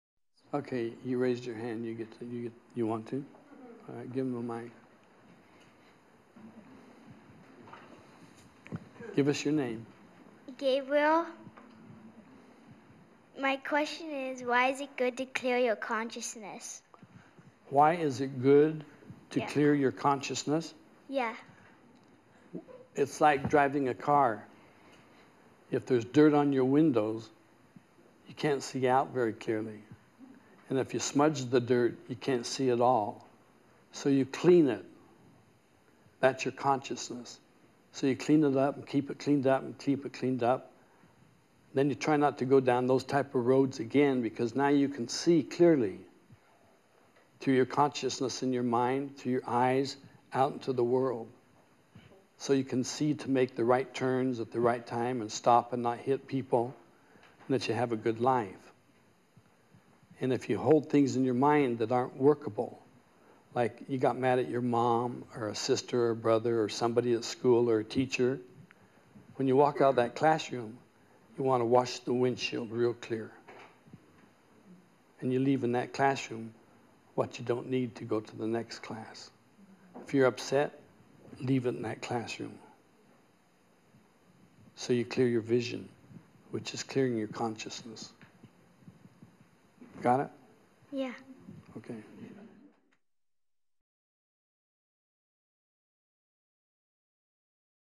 MSIA | John-Roger The Value of Clearing your Consciousness
MSIA Founder John-Roger on how to see more clearly

MPES152   A Moment of Peace: Why Clear Your Consciousness?
RUN TIME: 1:38

John-Roger, DSS

J-R:  Okay.  You raised your hand.  You get to if you want to?  All right.  Give him a mic.  Give us your name.

Gabriel Marvis: Gabriel. My question is, why is it good to clear your consciousness?

J-R:  Why is it good to clear your consciousness?

Gabriel:  Yeah.

J-R:  It’s like driving a car if there’s dirt on your windows, you can’t see out very clearly, and if you smudge the dirt, you can’t see at all so you clean it.  That’s your consciousness so you clean it up keep it cleaned up and keep it cleaned up and then you try not to go down those type of roads again because now you can see clearly through your consciousness in your mind, through your eyes, out into the world.  So you can see to make the right turns at the right time and stop and not hit people and that you have a good life.  And if you hold things in your mind that aren’t workable like you got mad at your mom or a sister or a brother or somebody at school or a teacher when you walk out of that classroom, you want to wash the windshield real clear.  And you leave in that classroom what you don’t need to go to the next class.  If you’re upset leave it in that classroom.  So you clear your vision, which is clearing your consciousness.  Got it?

Gabriel:  Yeah.

J-R:  Okay.